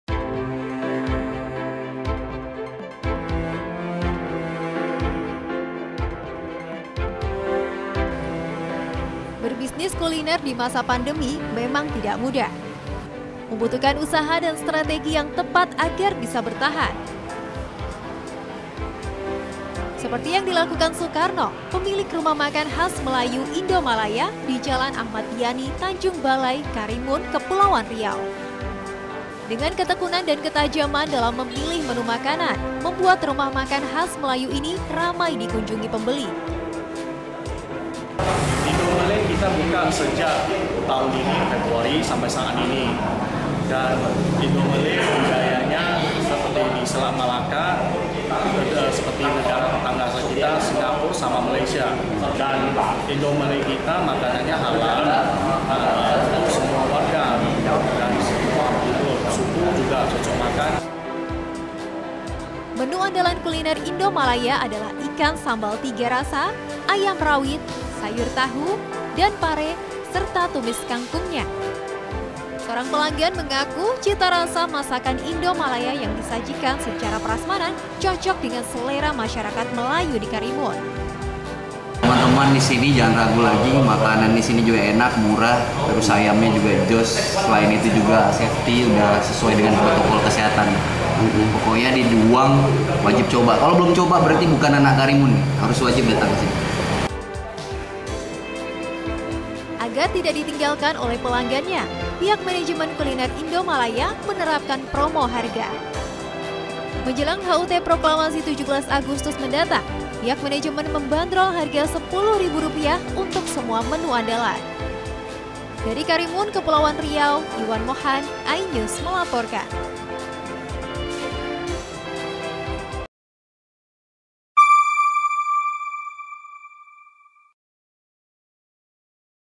Kuliner Khas Indonesia Malaya, Menu Andalan Ikan Sambal Tiga Rasa
Di tengah pandemi Covid-19, bisnis kuliner  menjadi sektor usaha yang masih diminati dan menjanjikan. Seperti yang dilakukan Sukarno, dan teman-temannya. Dengan keyakinanya mereka berhasil mengembangkan kuliner khas Melayu di tengah masa pandemi.